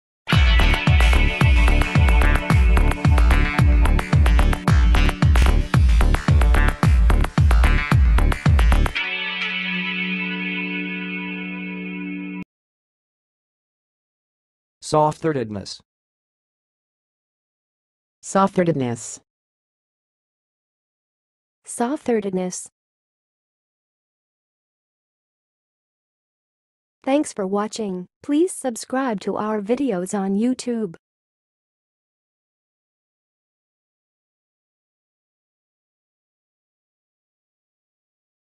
How to say "softheartedness"! (High Quality Voices)
Watch in this video how to say and pronounce "softheartedness"!